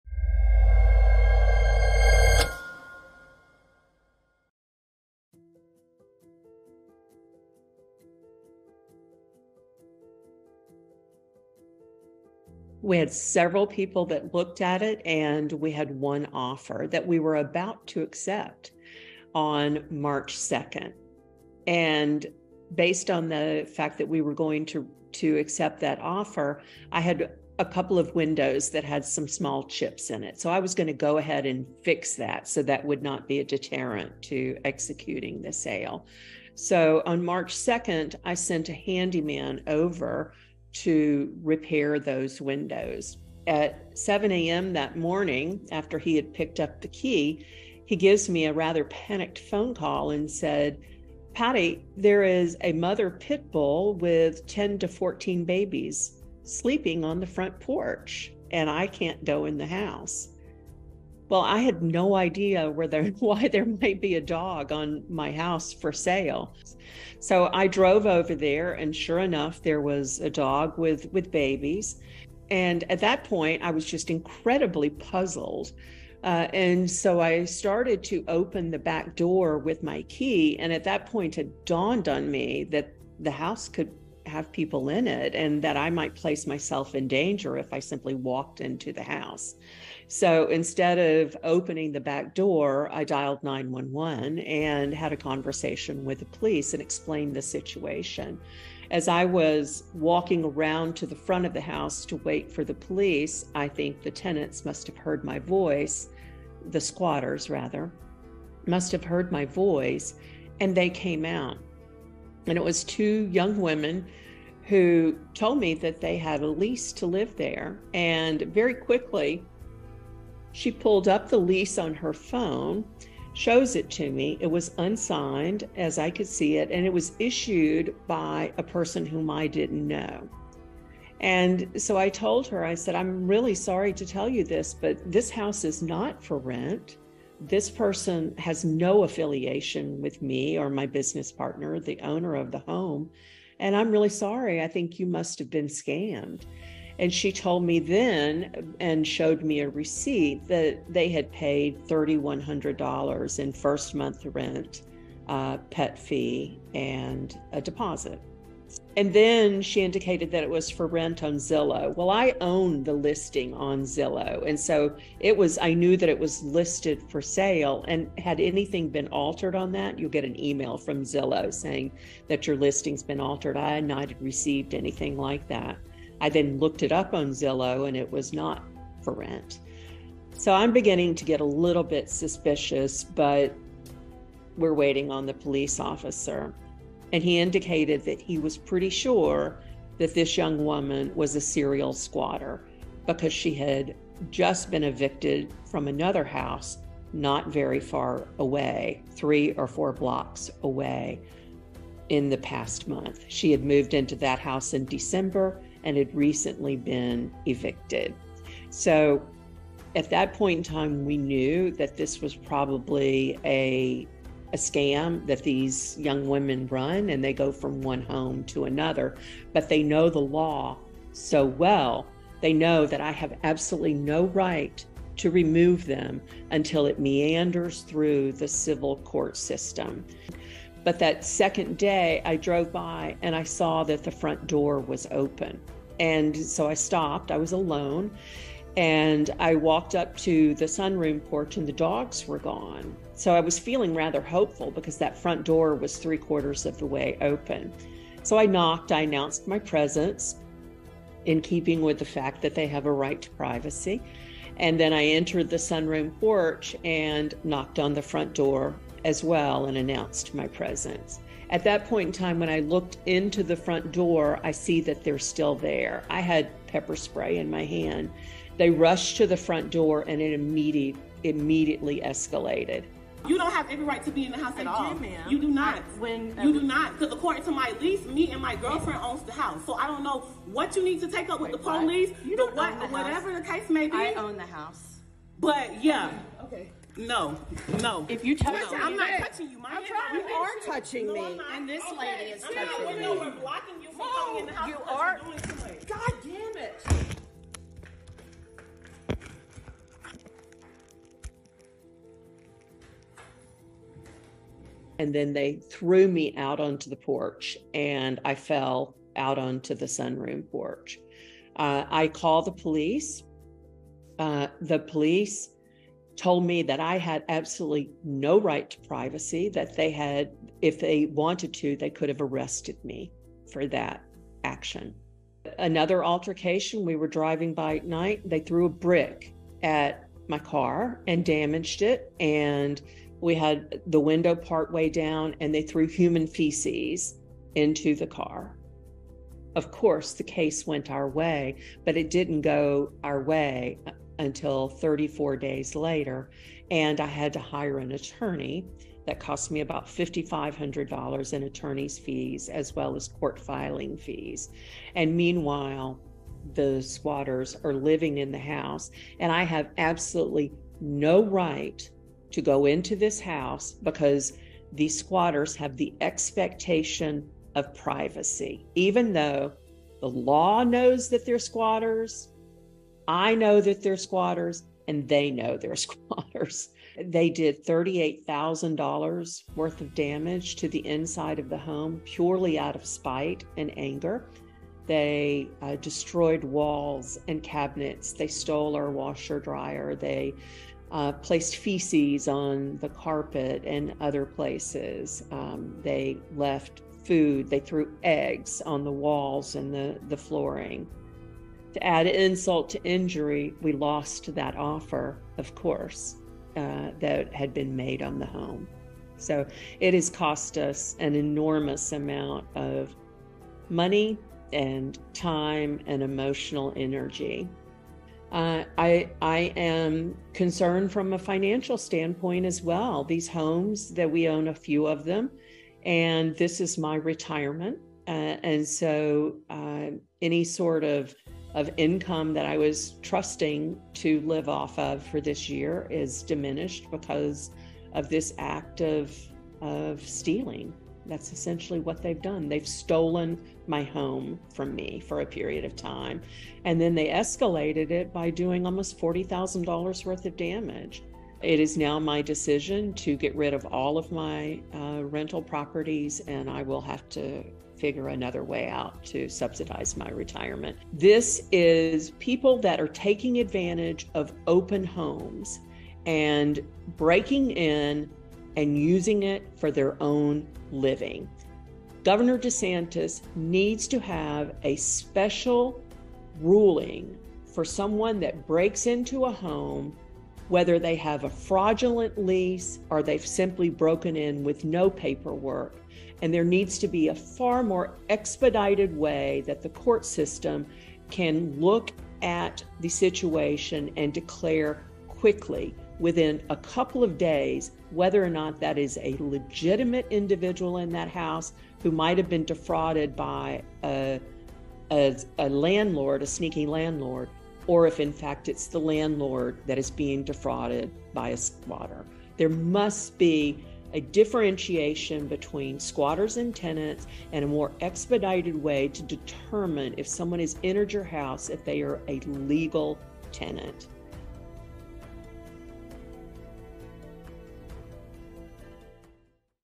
SERIAL SQUATTERS STOLE MY HOUSE: Landlord says people sat on property for month | Digital Original
After squatters illegally moved into a Jacksonville, Florida home and trashed it for more than 30 days, the owners decided they have had enough of being landlords.